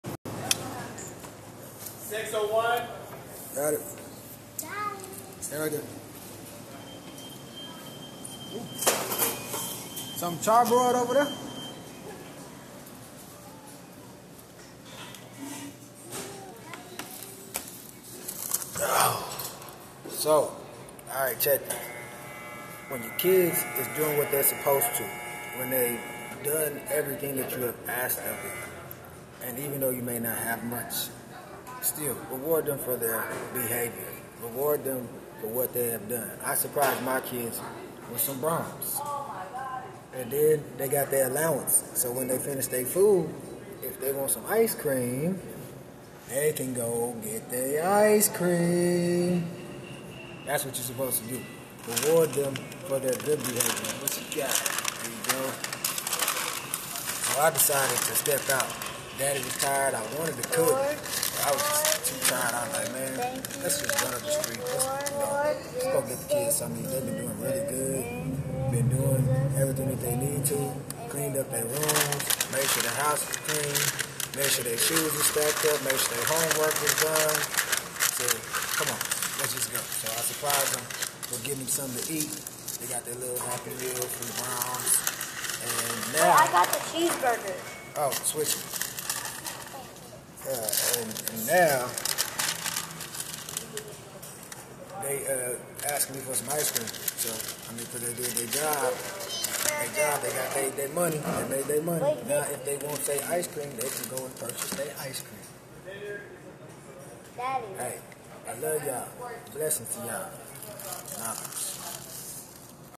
Kids get rewarded for good behavior
They earned their allowance and also a night out of eating because daddy was too tired to cook.